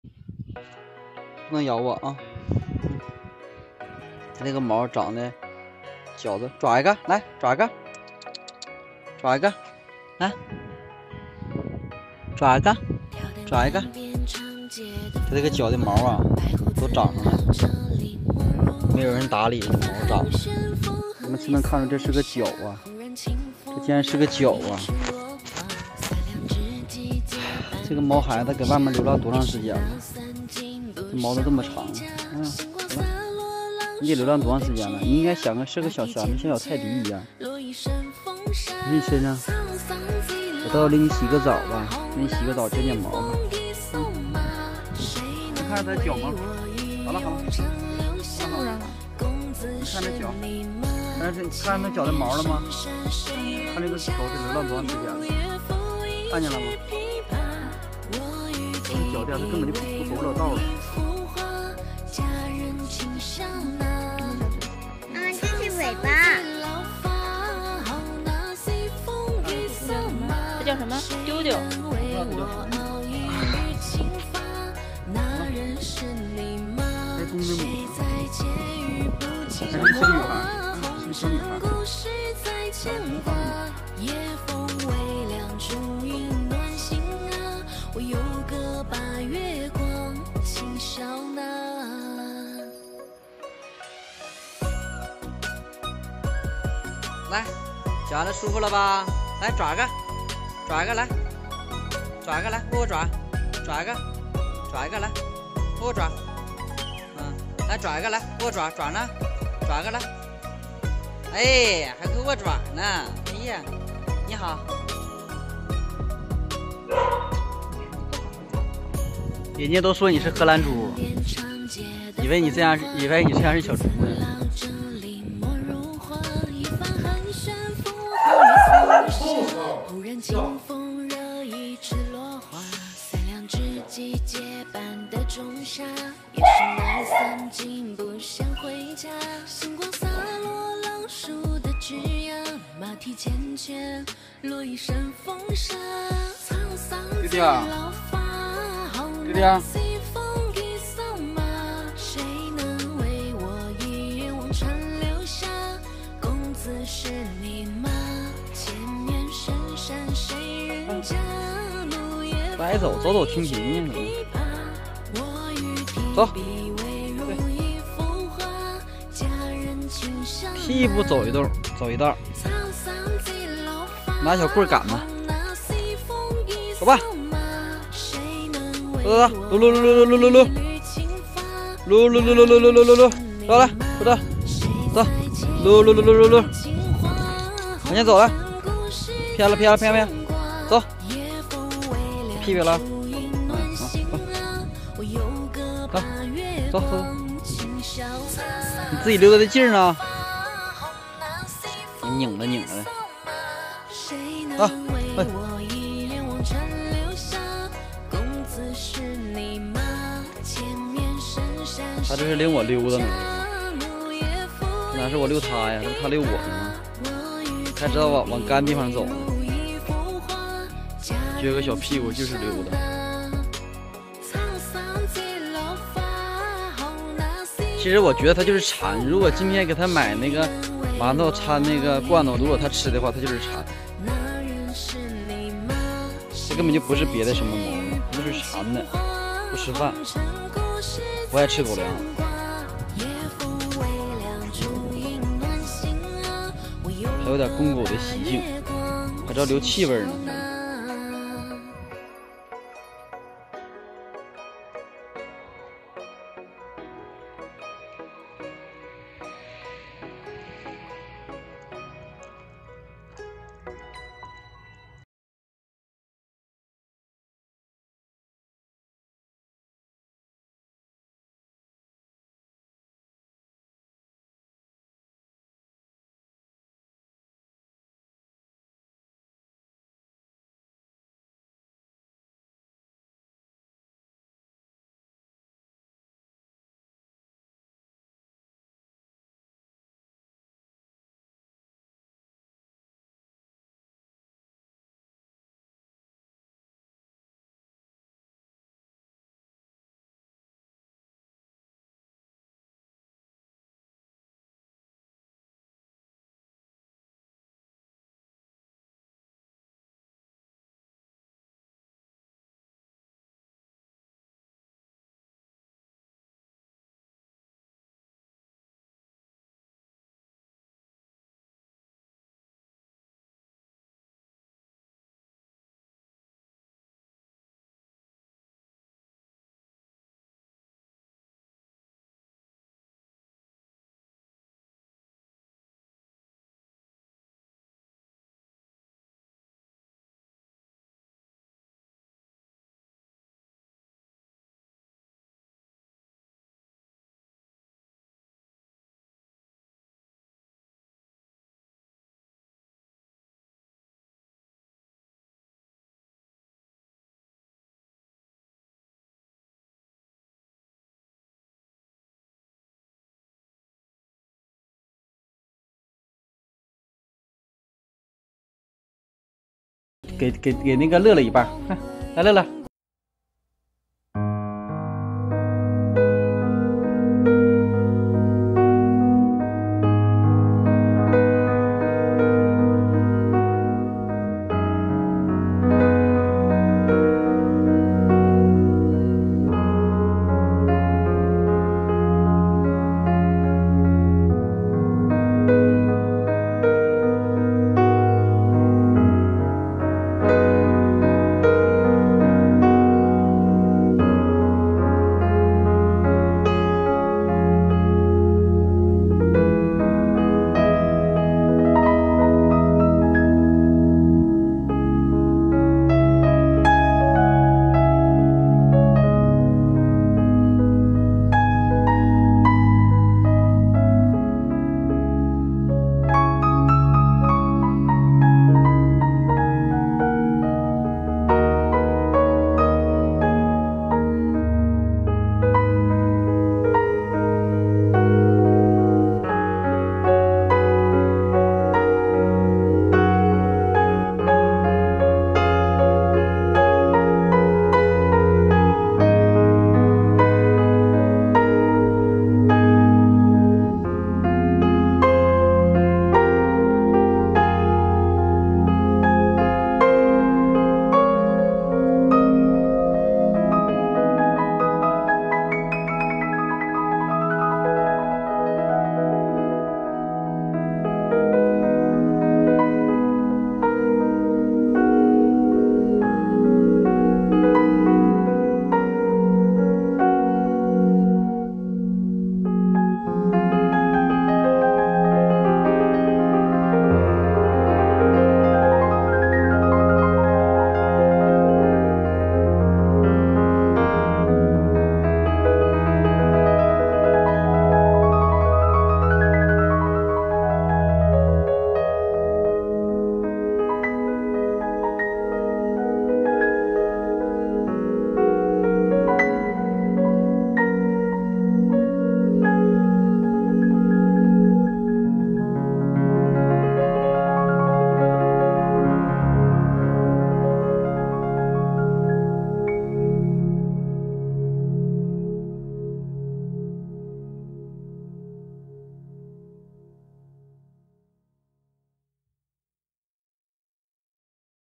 20201117—Dog Rescue in China狗狗离开了第二天，那种失去的痛，只有养过狗的人才能体会Day2 after DiuDiu passed away. The pain
only those people who once had a dog would experience.

关爱生命，从救助身边的每一只流浪狗做起。东北小伙小迟，和您分享他与流浪狗的故事。

西瓜视频：小迟和流浪狗的故事